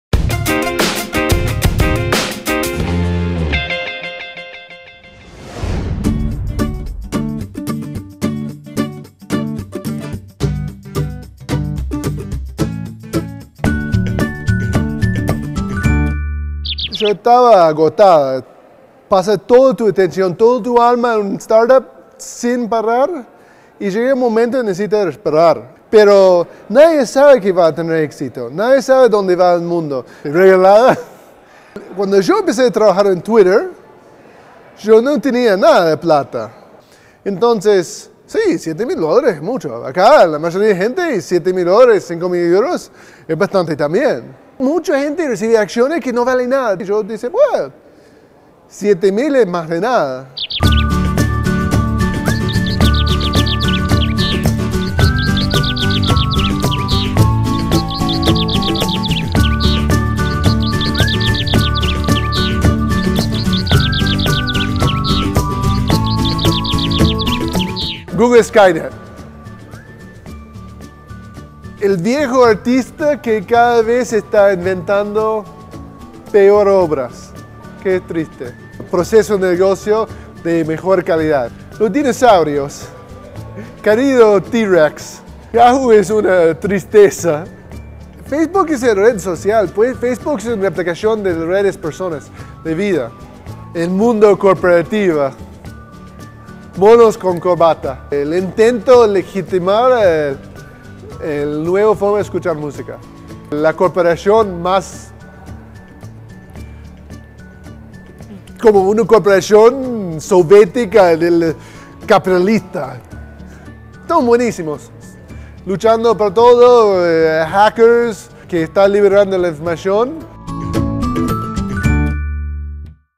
Entrevista con Evan Henshaw-Plath, cofundador de Twitter
Hemos charlado con Evan Henshaw-Plath, cofundador de Twitter, que abandonó la plataforma justo antes de que despegara y empezara a convertirse en lo que es hoy.